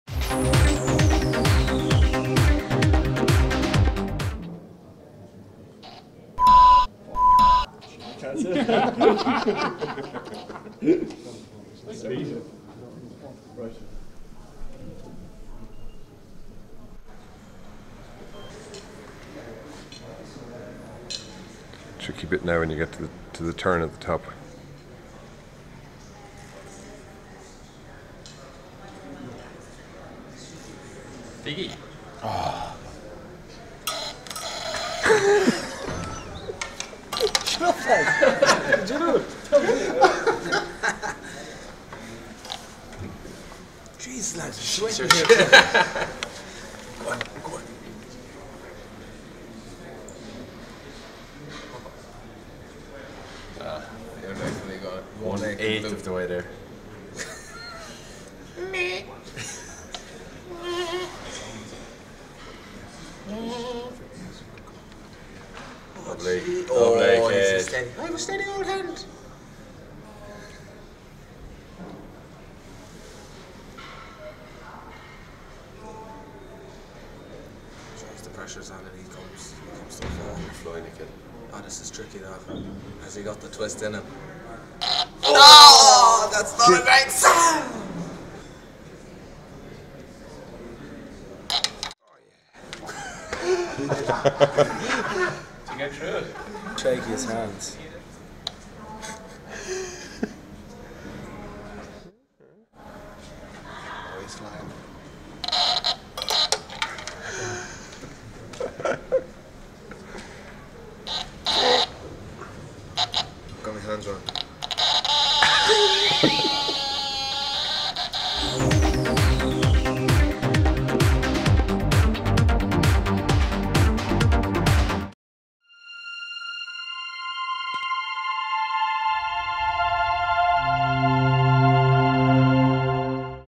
Irish Rugby TV: You're Wreckin' Me Buzz
It takes skill, concentration and a steady hand and the lads definitely got a buzz out of this one. Peter O'Mahony, Jordi Murphy, Greg Feek, Jason Cowman and Sean O'Brien tried their hand but did anyone make it clear?